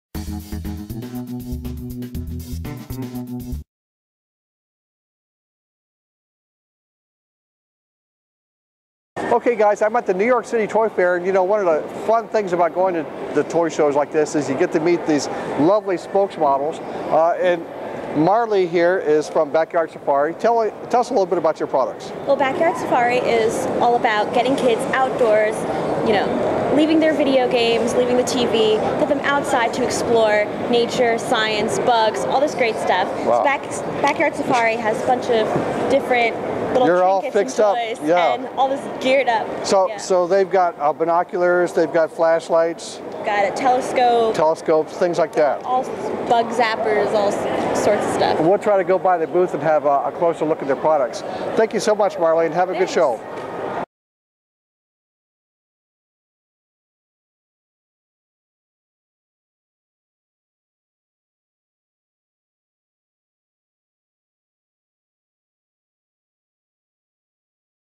Backyard Safari Science and Nature Toys 2011 Toy Fair Preview
Backyard Safari toys are all about getting you out of the house and exploring the natural world. Join professional spokes-model Marlee Roberts as she talks about some of the adventure toys from Backyard Safari. Learn about nature and science with binoculars, flashlights, telescopes, bug catchers and more!